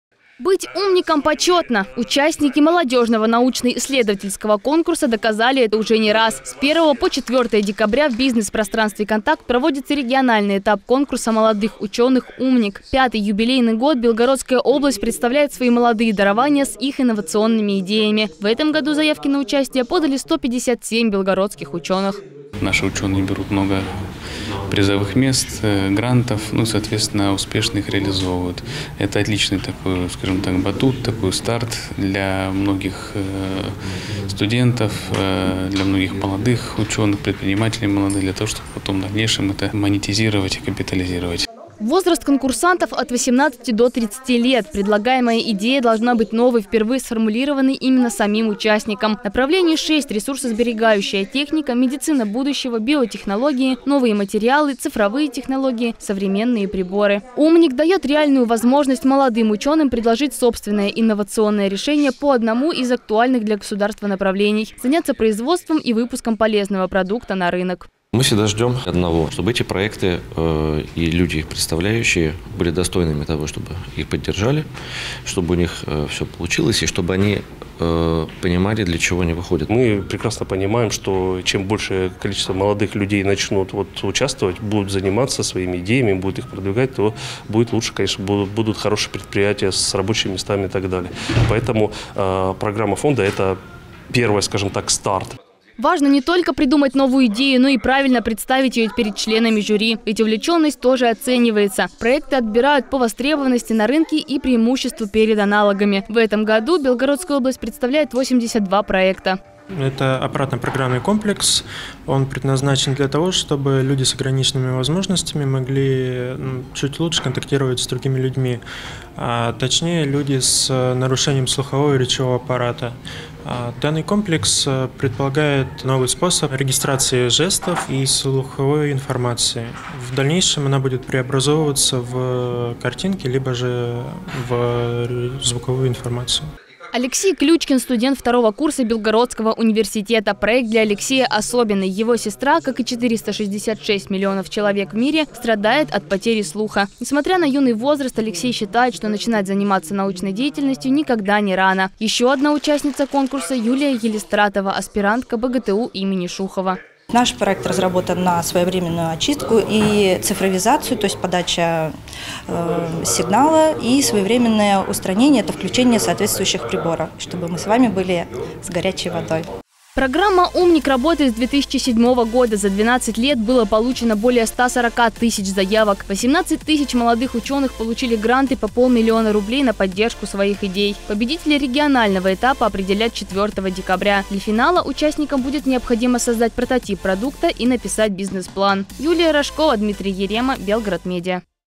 157 белгородских учёных поборются за 500 000 рублей
С 1 по 4 декабря в бизнес — пространстве «Контакт» проводится региональный этап конкурса молодых учёных «УМНИК»